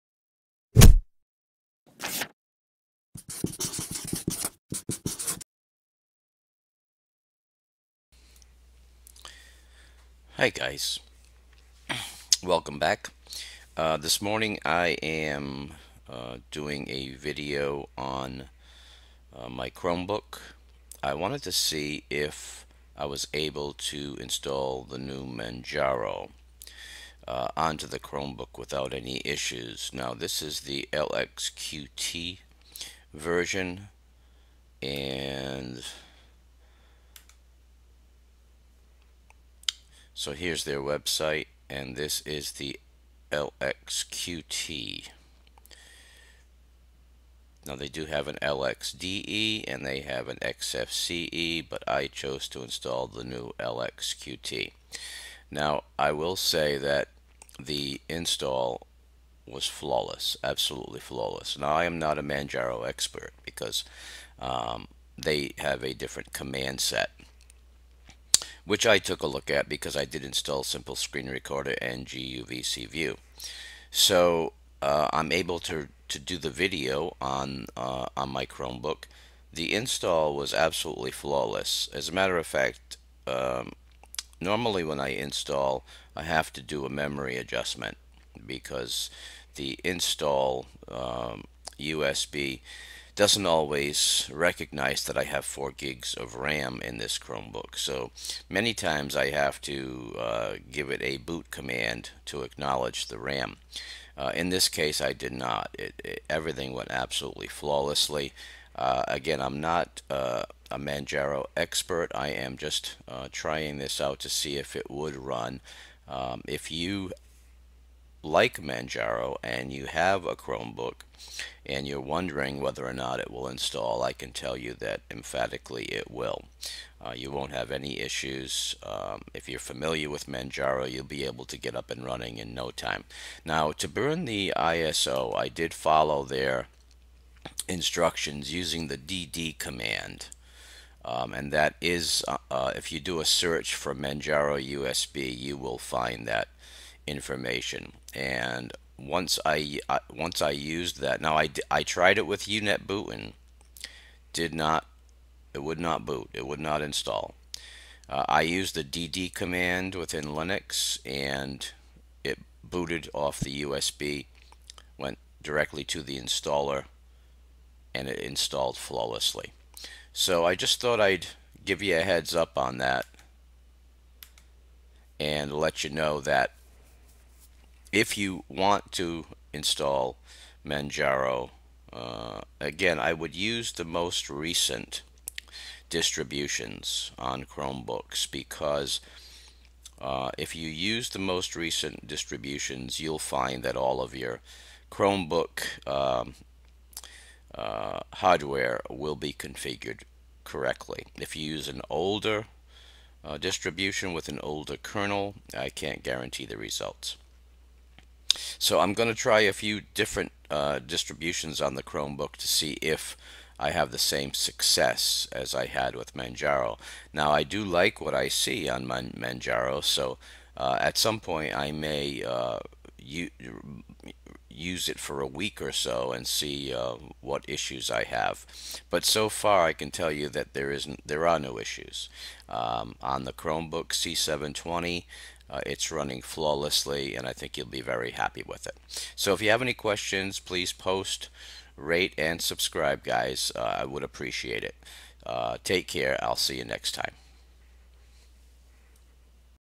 Manjaro Linux on Chromebook C720
Hi Guys!  This is a short video showing a successful install of Manjaro Lxqt on my C720 Chromebook.